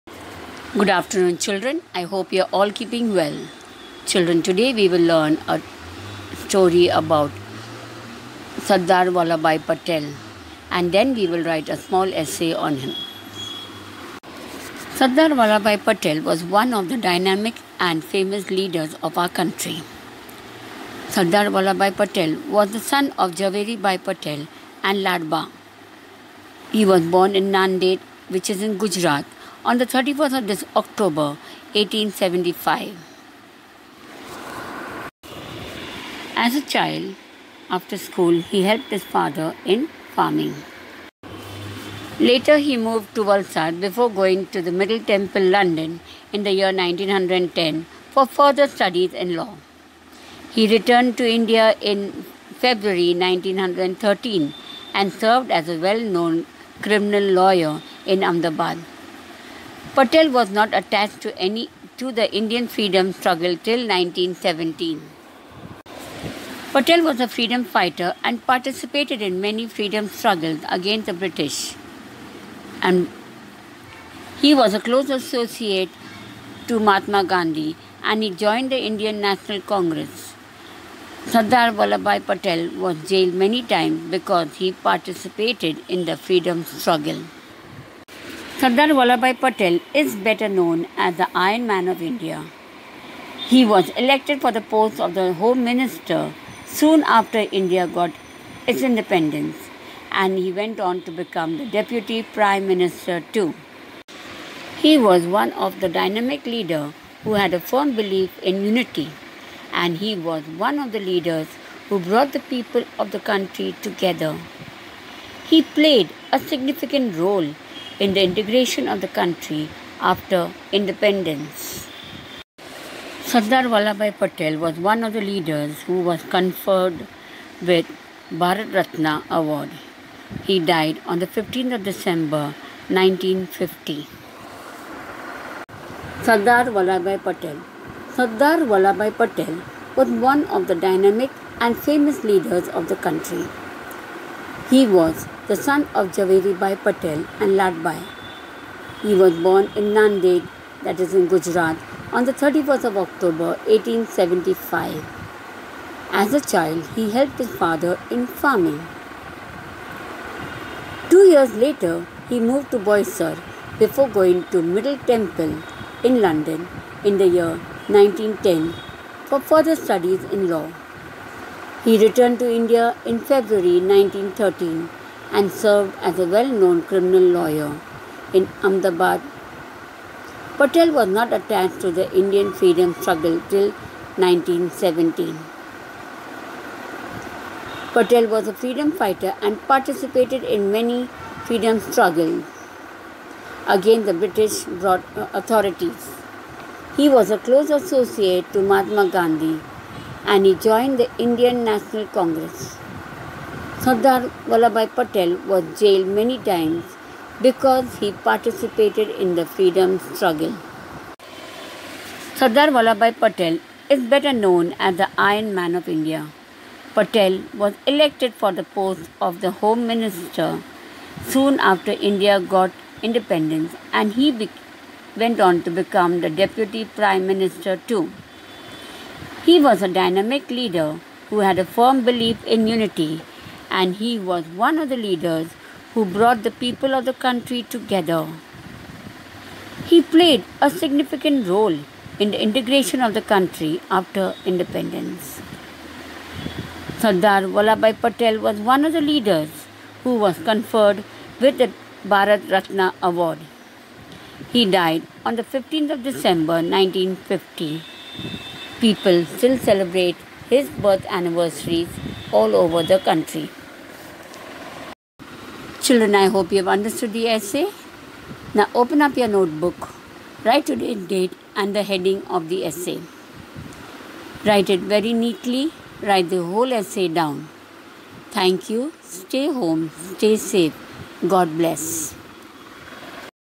Sadar Vallabhbhai Patel By Mrs. Andrea Mary A Lobo
An essay on Sadar Vallabhbhai Patel is taught to the kids. This is done to commemorate the birth anniversary of our leader Sardar Vallabhbhai Patel